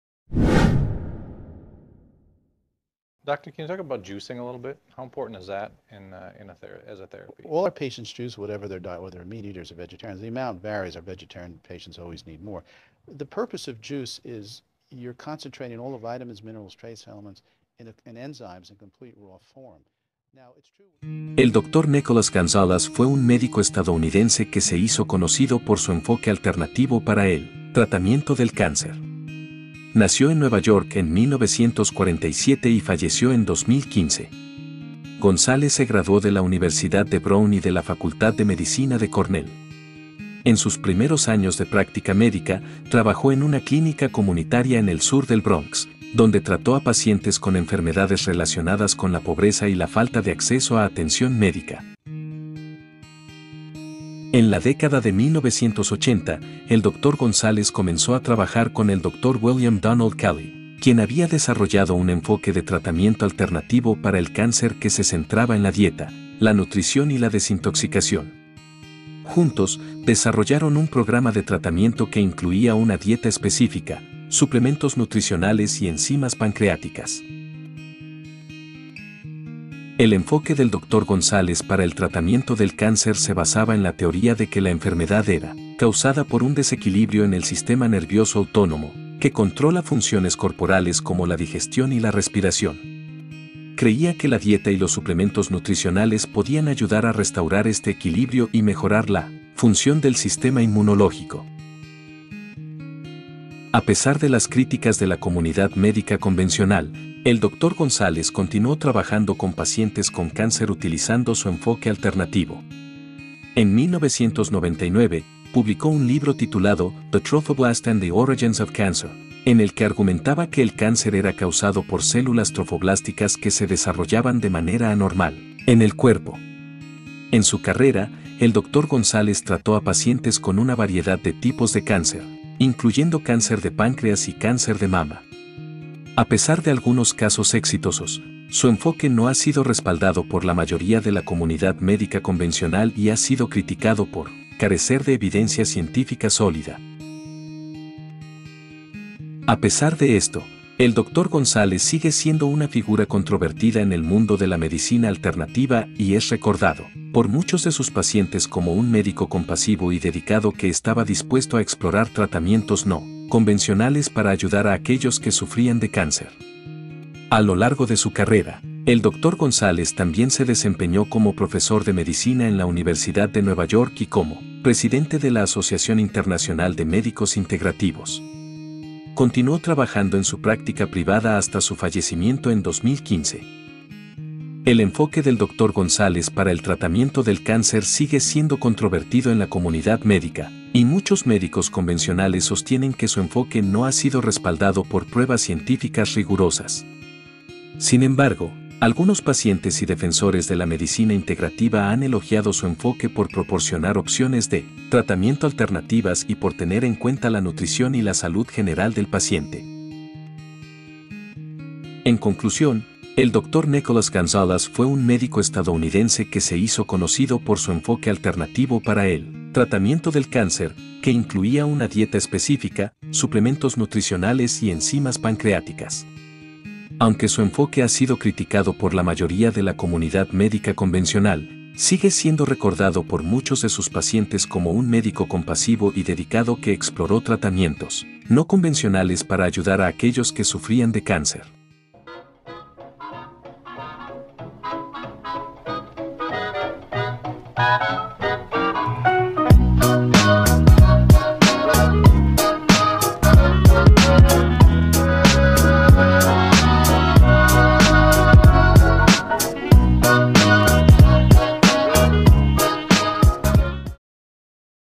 Dr. Nicolas Gonzalez Documentary
El Dr. Nicholas Gonzalez fue un médico estadounidense que se hizo conocido por su enfoque alternativo para el tratamiento del cáncer. Nació en Nueva York en 1947 y falleció en 2015.

Gonzalez se graduó de la Universidad de Brown y de la Facultad de Medicina de Cornell. En sus primeros años de práctica médica, trabajó en una clínica comunitaria en el sur del Bronx, donde trató a pacientes con enfermedades relacionadas con la pobreza y la falta de acceso a atención médica.

En la década de 1980, el Dr. Gonzalez comenzó a trabajar con el Dr. William Donald Kelley, quien había desarrollado un enfoque de tratamiento alternativo para el cáncer que se centraba en la dieta, la nutrición y la desintoxicación. Juntos, desarrollaron un programa de tratamiento que incluía una dieta específica, suplementos nutricionales y enzimas pancreáticas.

El enfoque del Dr. Gonzalez para el tratamiento del cáncer se basaba en la teoría de que la enfermedad era causada por un desequilibrio en el sistema nervioso autónomo, que controla funciones corporales como la digestión y la respiración. Creía que la dieta y los suplementos nutricionales podían ayudar a restaurar este equilibrio y mejorar la función del sistema inmunológico.

A pesar de las críticas de la comunidad médica convencional, el Dr. Gonzalez continuó trabajando con pacientes con cáncer utilizando su enfoque alternativo. En 1999, publicó un libro titulado "The Trophoblast and the Origins of Cancer", en el que argumentaba que el cáncer era causado por células trofoblásticas que se desarrollaban de manera anormal en el cuerpo.

En su carrera, el Dr. Gonzalez trató a pacientes con una variedad de tipos de cáncer, incluyendo cáncer de páncreas y cáncer de mama. A pesar de algunos casos exitosos, su enfoque no ha sido respaldado por la mayoría de la comunidad médica convencional y ha sido criticado por carecer de evidencia científica sólida.

A pesar de esto, el Dr. Gonzalez sigue siendo una figura controvertida en el mundo de la medicina alternativa y es recordado por muchos de sus pacientes como un médico compasivo y dedicado que estaba dispuesto a explorar tratamientos no convencionales para ayudar a aquellos que sufrían de cáncer.

Continúa

A lo largo de su carrera, el Dr. Gonzalez también se desempeñó como profesor de medicina en la Universidad de Nueva York y como presidente de la Asociación Internacional de Médicos Integrativos. Continuó trabajando en su práctica privada hasta su fallecimiento en 2015.

El enfoque del Dr. Gonzalez para el tratamiento del cáncer sigue siendo controvertido en la comunidad médica, y muchos médicos convencionales sostienen que su enfoque no ha sido respaldado por pruebas científicas rigurosas. Sin embargo, algunos pacientes y defensores de la medicina integrativa han elogiado su enfoque por proporcionar opciones de tratamiento alternativas y por tener en cuenta la nutrición y la salud general del paciente.

En conclusión, el Dr. Nicholas Gonzalez fue un médico estadounidense que se hizo conocido por su enfoque alternativo para el tratamiento del cáncer, que incluía una dieta específica, suplementos nutricionales y enzimas pancreáticas. Aunque su enfoque ha sido criticado por la mayoría de la comunidad médica convencional, sigue siendo recordado por muchos de sus pacientes como un médico compasivo y dedicado que exploró tratamientos no convencionales para ayudar a aquellos que sufrían de cáncer.